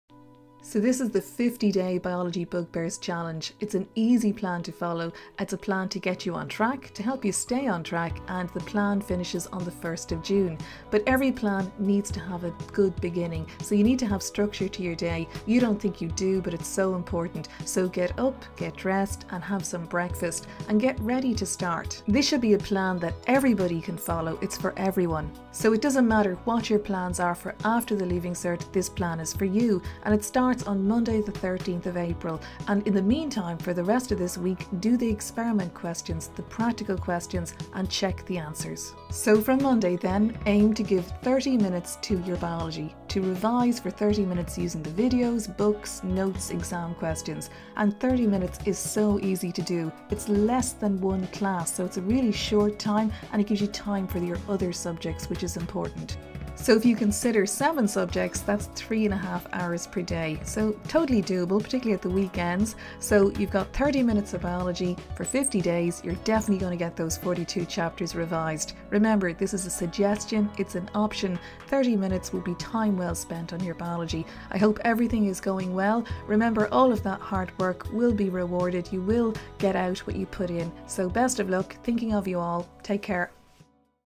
50 Day Biology Leaving Cert Challenge
This is just a plan I thought of to help those of you who are getting fed up with all the leaving cert chat. This is an option only and intended to help my students.